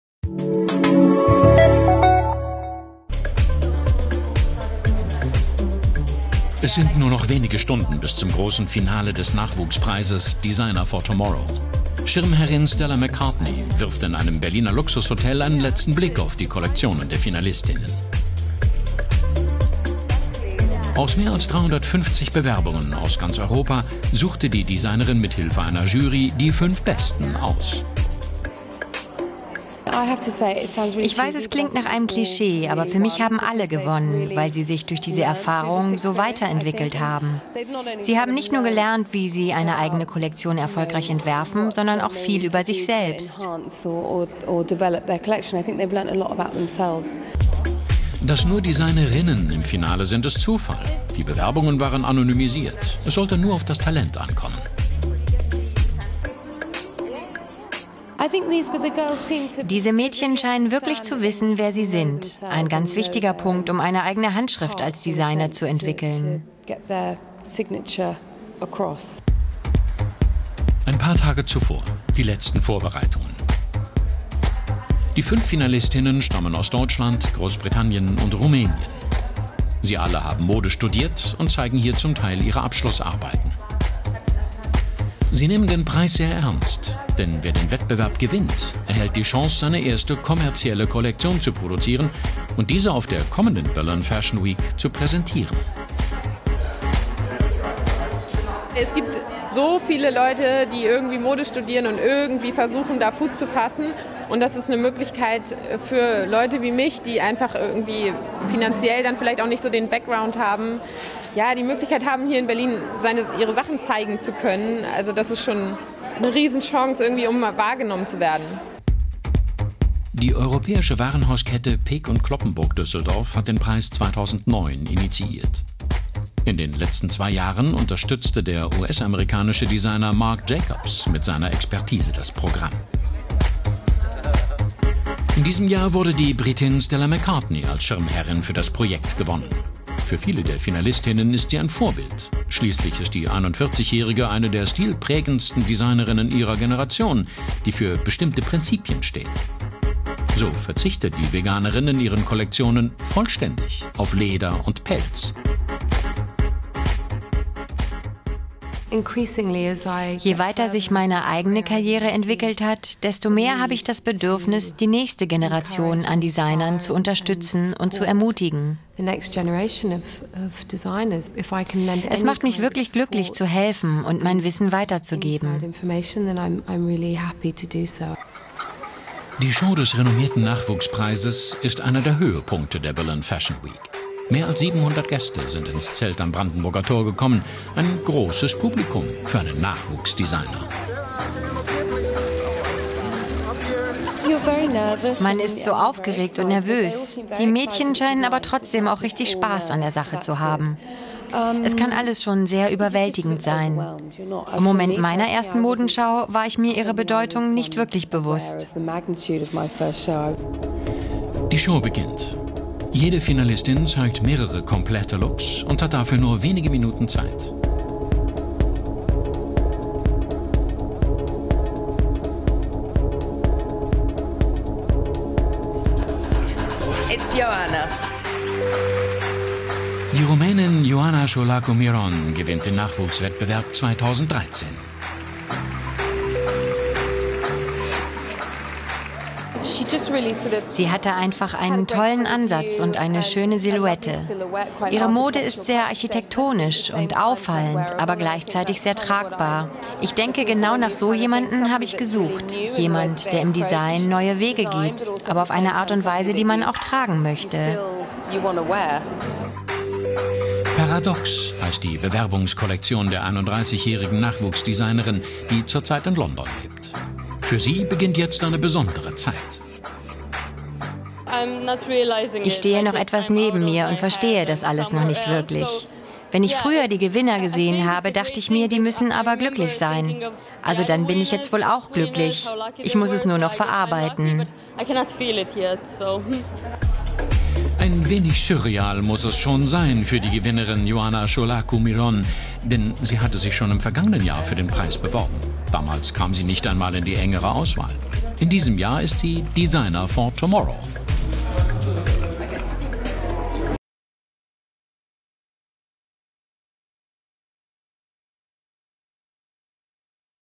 Stella McCartney sucht Nachwuchsdesigner | Euromaxx
Im Rahmen der Fashion Week in Berlin wird der "Designer for Tomorrow" gesucht. Nachwuchsdesigner aus ganz Europa haben sich angemeldet. 2013 ist die Schirmherrin des Wettbewerbs die britische Star-Designerin Stella McCartney.